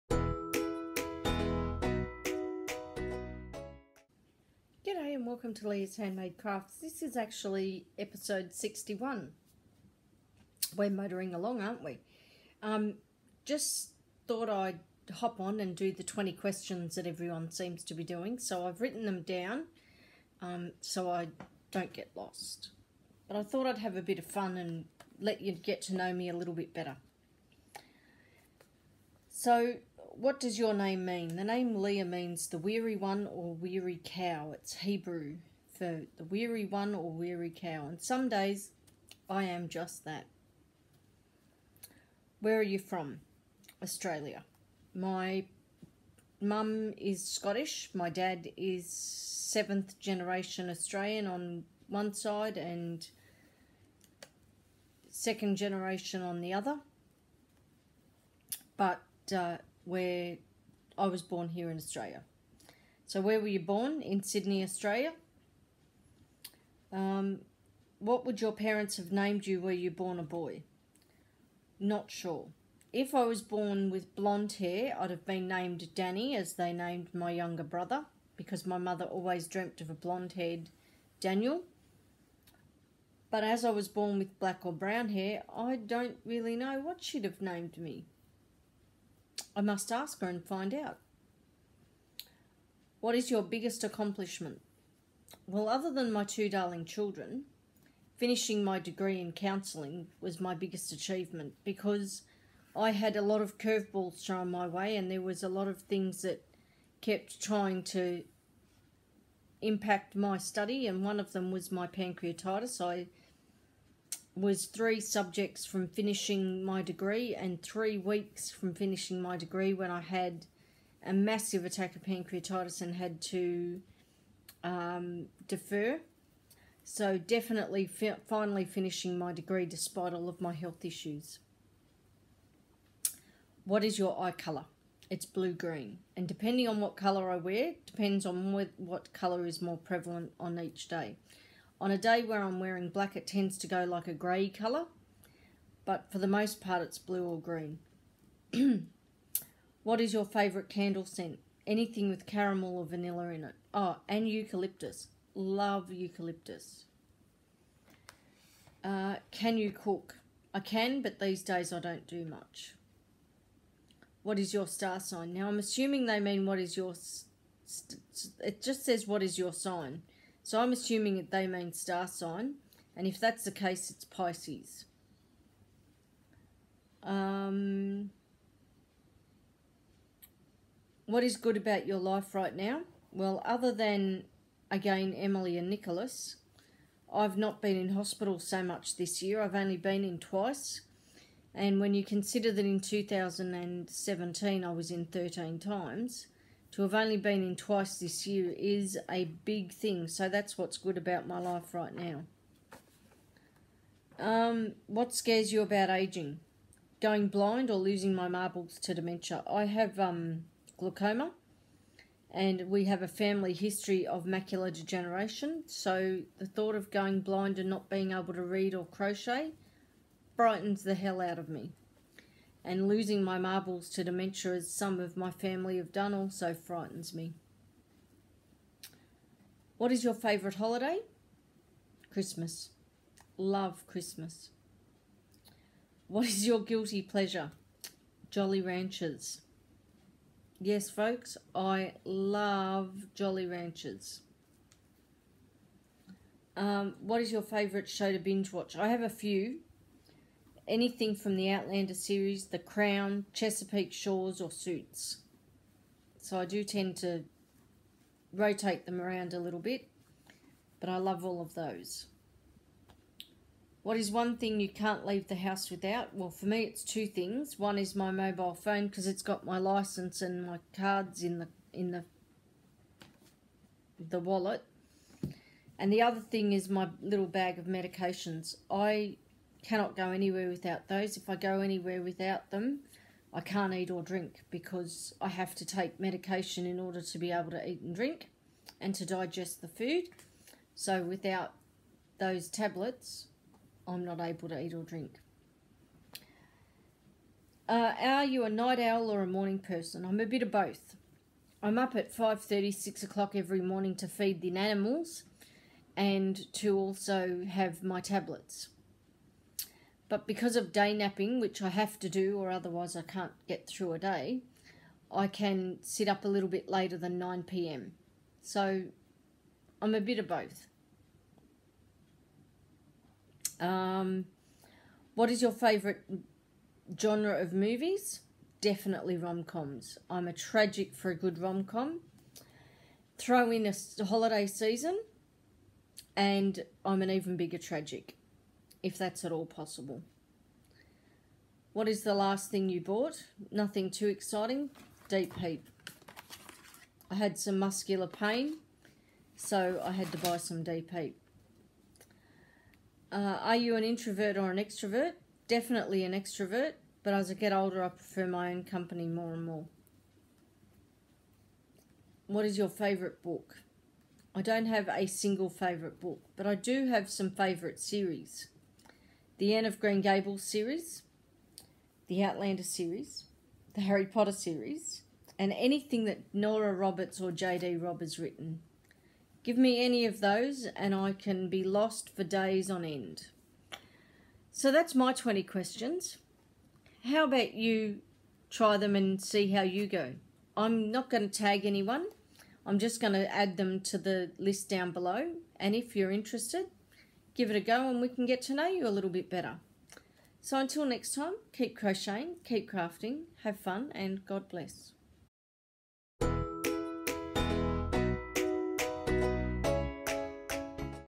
Leah's Handmade Crafts
20 Questions
1. What does your name mean?
2. Where are you from?
3. Where were you born?
4. What would your parents have named you if you had been the other sex?
5. What is your biggest accomplishment?
6. What is your eye color?
7. What is your favorite candle scent?
8. Can you cook?
9. What is good about your life right now?
10. What is your sign?
11. What scares you about aging?
12. What is your favorite holiday?
13. What is your guilty pleasure?
14. What is your favorite show to binge watch?
15. What is one thing you can’t leave the house without?
16. Are you a morning person or a night owl
17. What is your favorite genre of movie?
18. What is the last thing you bought?
19. Are you an introvert or an extrovert?
20. What is your favorite book?

Or blog... it’s a bit of a work in progress

Or drop me a line at
Moorebank NSW 1875
Australia.